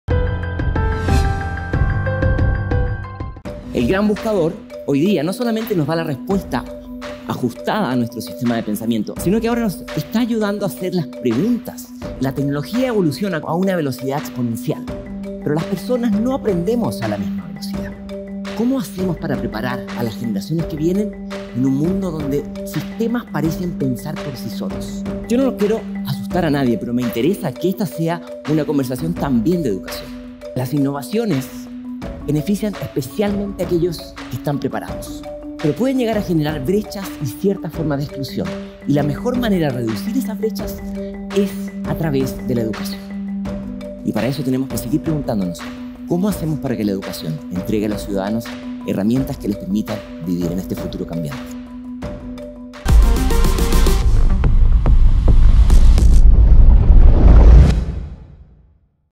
Charlas del Futuro 2020 | Cristóbal Cobo
Para afrontar los cambios del futuro, es importante educarnos en el presente y conocer hacia dónde vamos; la evolución de la tecnología progresa a pasos agigantados, y no queremos quedarnos atrás. ¿Cómo haremos para que la educación entregue a los ciudadanos, herramientas que le permitan vivir en este futuro cambiante?”, esta es la interrogante que plantea Cristobal Cobo, en una era tecnológica llena de innovaciones y desafíos.